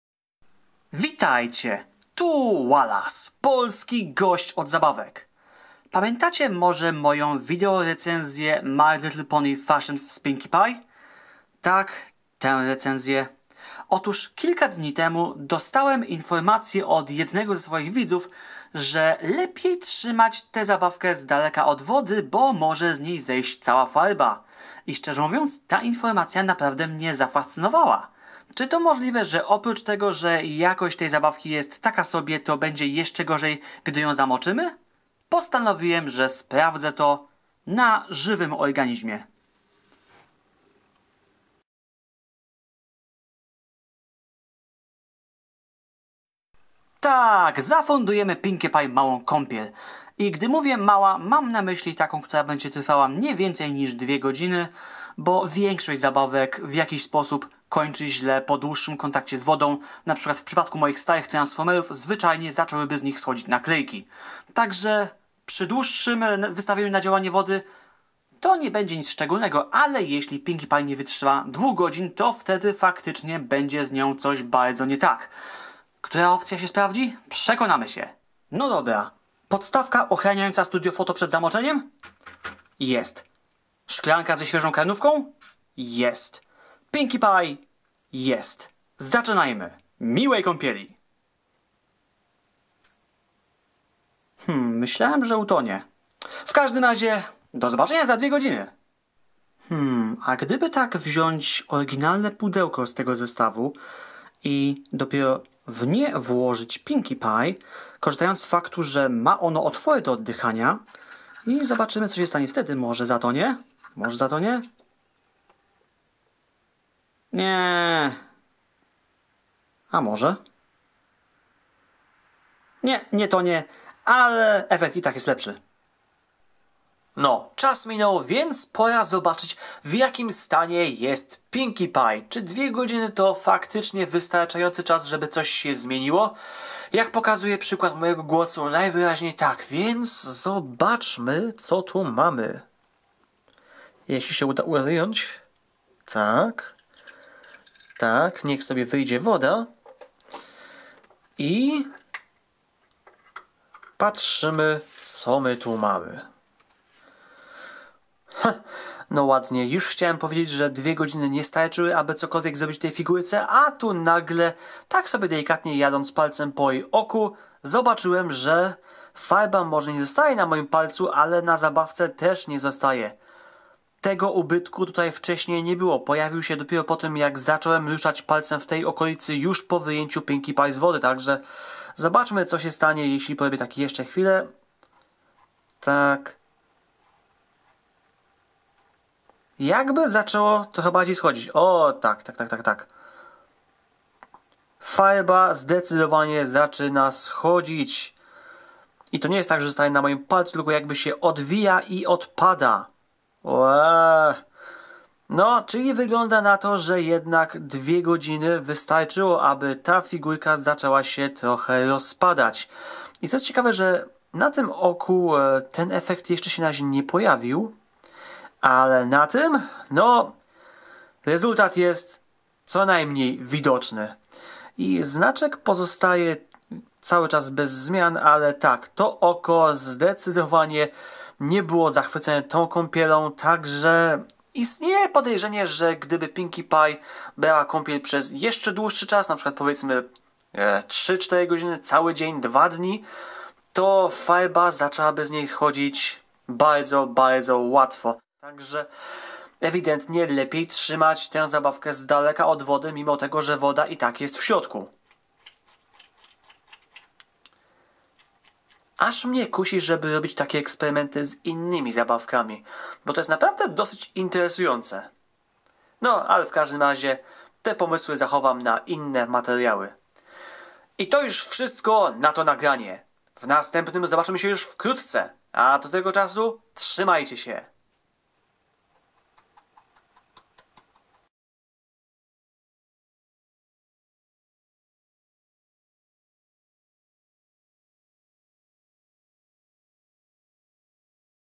Kucyk kontra woda - test zabawki My Little Pony Fash'ems | Recenzje Zabawek Wallasa
Podobno te zgniatalne kucyki kiepsko znoszą kontakt z wodą. Dziś sprawdzimy jak bardzo jest to problematyczne na żywym organizmie.

Jeśli to nagranie Wam się podoba, może mogę Was zainteresować i innymi z mojego dorobku?

Kucyk ( Pinkie Pie) kontra woda ( zwykła kranowa ) - test plastycznej / zgniatalnej zabawki My Little Pony Fash'ems od Tech 4 Kids | Recenzje Zabawek Wallasa